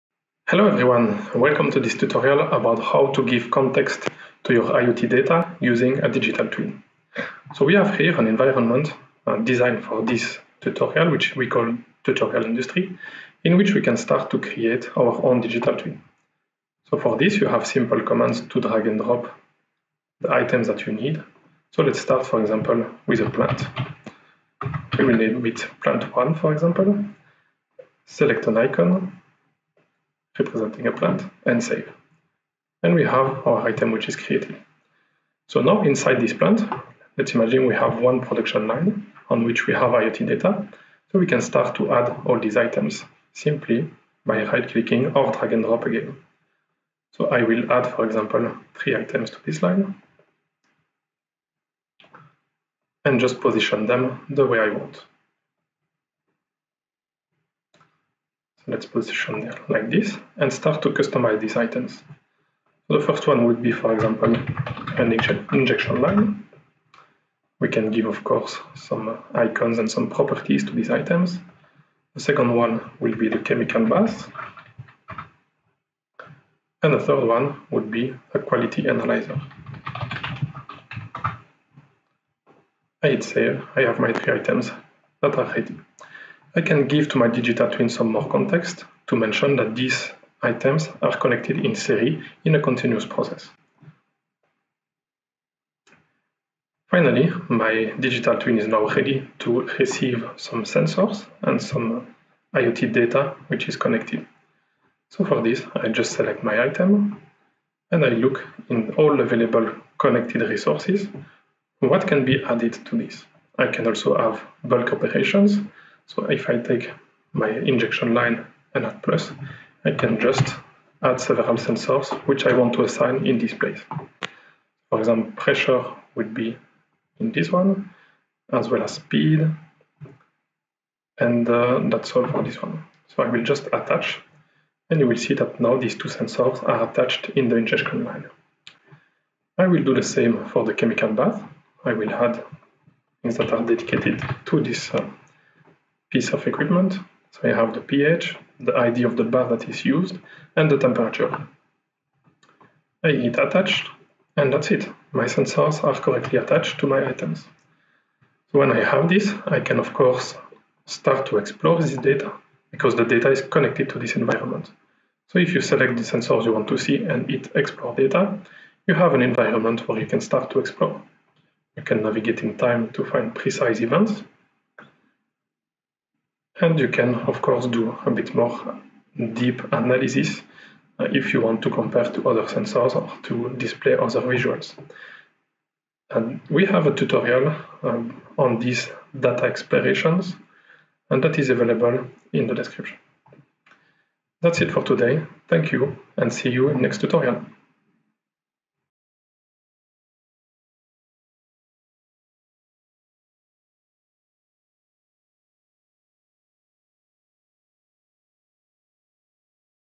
Tutorial | How to give context to IoT data with a Digital Twin
Here's how to contextualize IoT data with a Digital Twin. A Digital Twin is a digital representation of on-site physical or virtual data sources, that can be used as bi-directional interface between the users and connected devices. After a simple mapping of IoT data in the Digital Twin, interacting with processes becomes really easy.